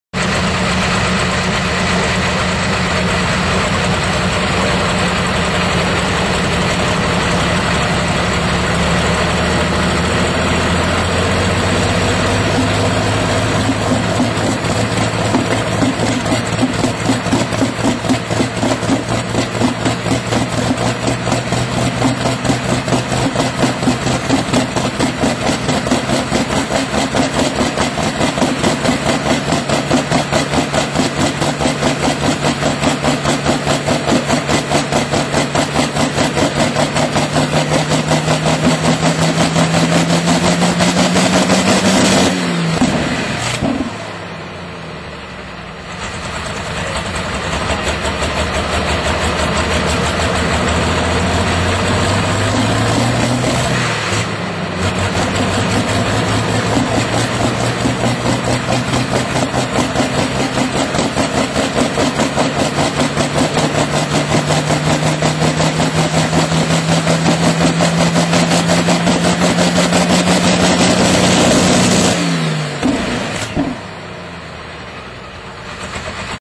REDNEX Cadzand 2010 test
REDNEX testing the waterinjection system